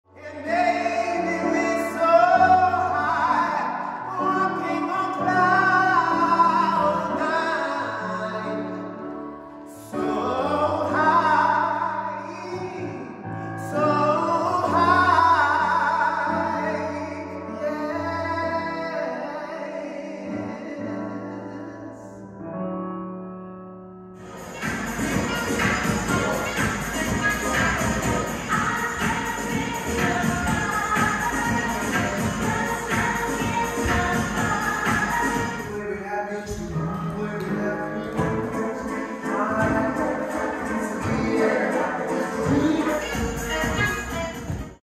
Nathan Henry (Short Reel)
Singing, Dancing reel for Nathan Henry represented by Christopher Antony Associates.